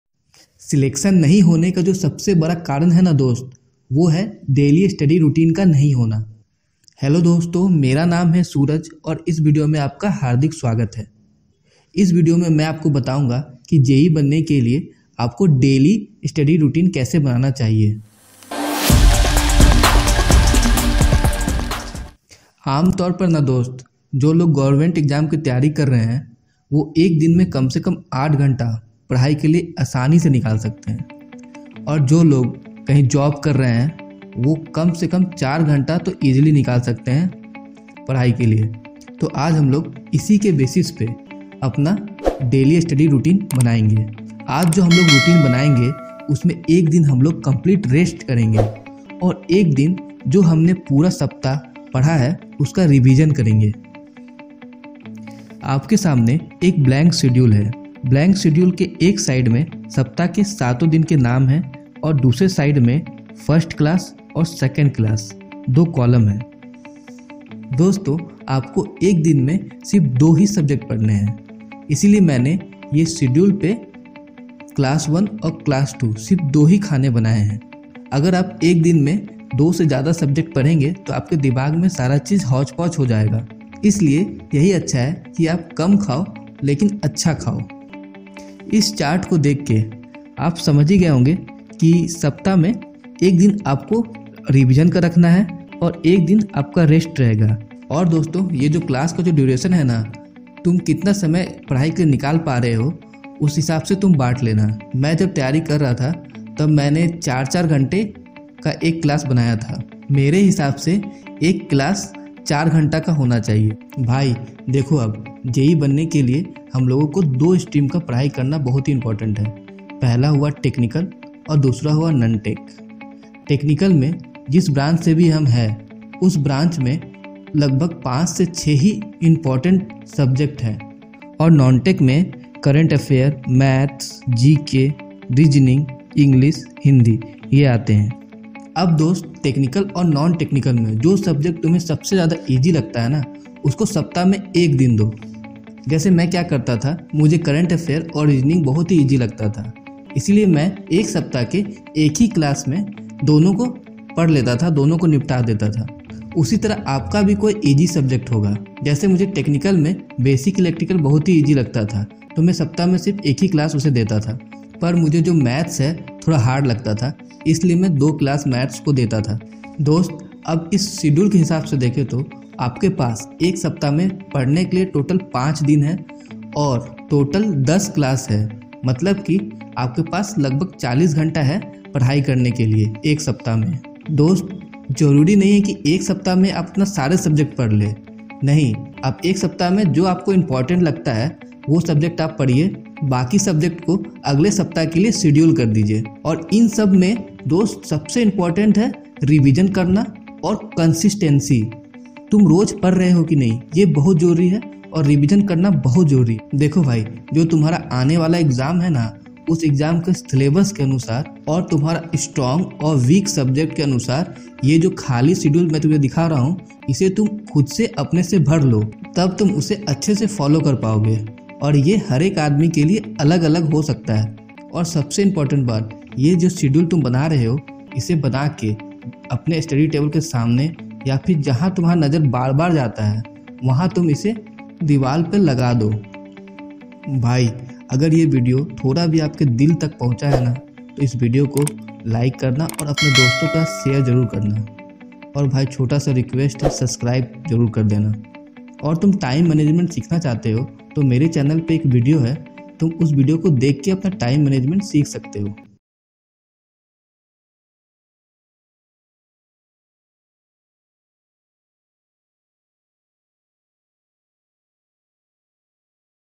100% SUCCESSFUL Study Routine For SSC JE/AE , UPPCL JE/ AE| Time Table For Study Junior Engineer
Bhai,
Ye video aapka help karega daily study routine banane me or jada consistency usko follow karne me.
Agar koi v help chahiye toh telegram group me freely bol sakte ho

aager video aacha laga toh help kar do bhai

Thanks & Regards
Topic-100% SUSCESSFUL Study Routine For SSC JE/AE , UPPCL JE/ AE| Time Table For Study  Junior Engineer / ASISTANT ENGINEER